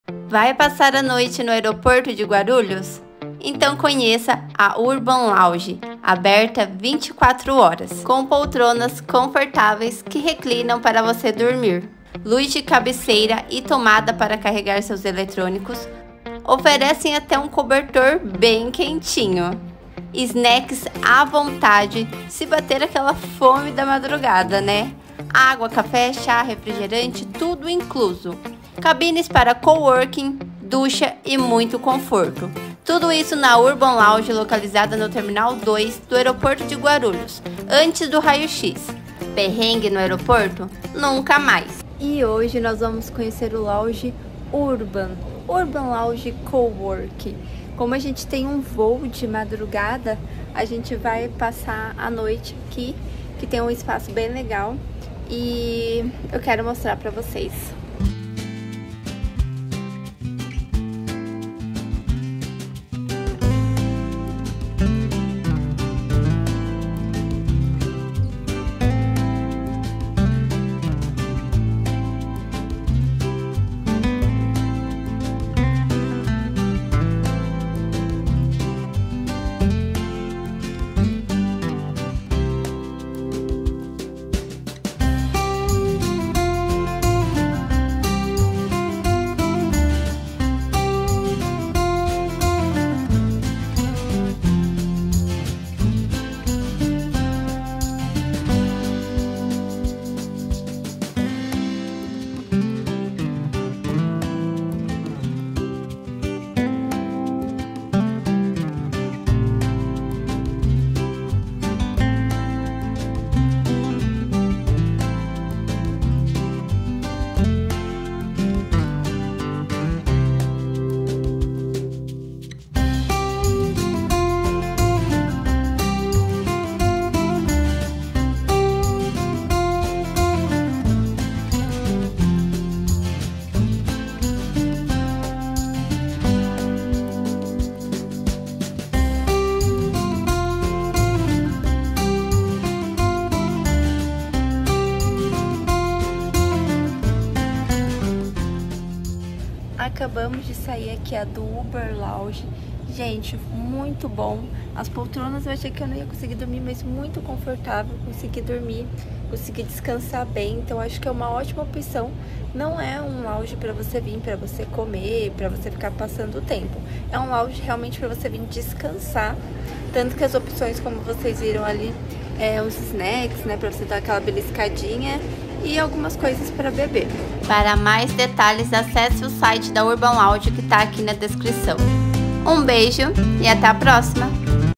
Onde dormir no AEROPORTO DE GUARULHOS | URBAN LOUNGE
Urban Lounge Cowork no aeroporto de Guarulhos, uma excelente opção pra quem não sabe onde dormir no aeroporto de Guarulhos.

✅Cartão Internacional Nomad, ganhe até $20 dólares de cashback no primeiro câmbio. Cupom (EUQUEROCONHECER)

🙋🏼‍♀️Quer me conhecer mais?